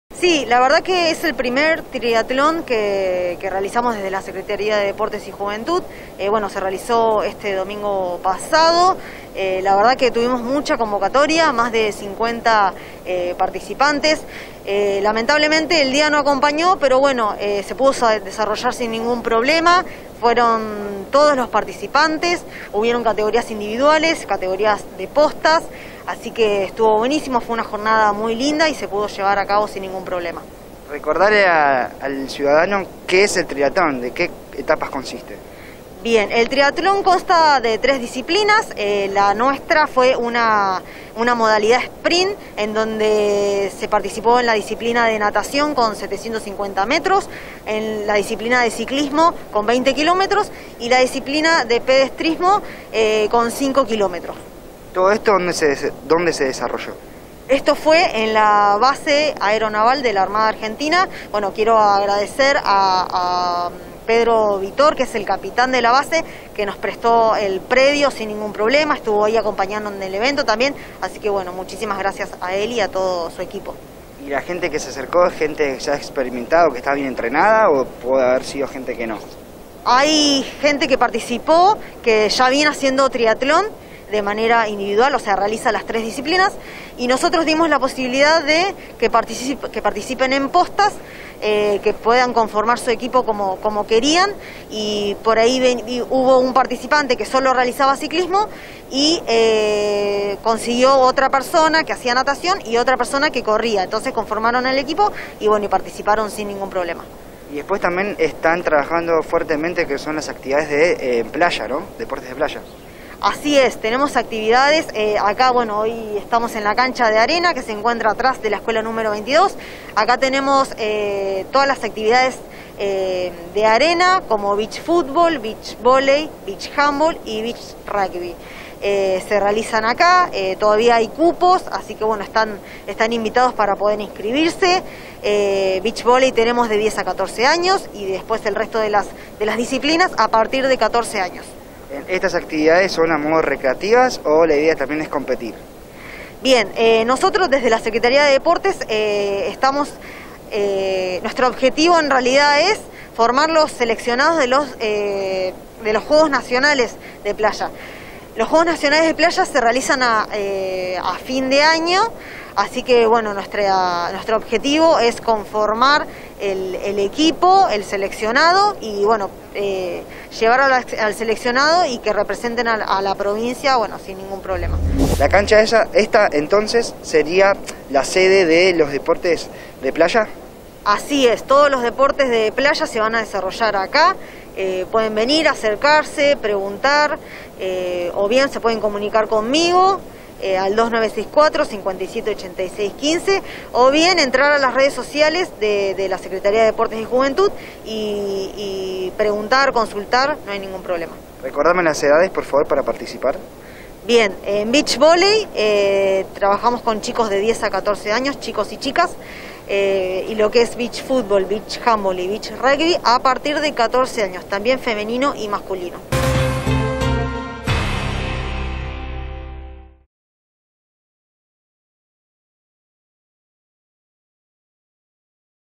LLEVAN ADELANTE ACTIVIDADES DE DEPORTES DE PLAYA.
Desde la Secretaria de Deportes y Juventud el domingo realizaron el primer triatlón del año. En la cancha de arena de la Escuela 22 se lleva a cabo vóley, fútbol, Handball y rugby de Playa.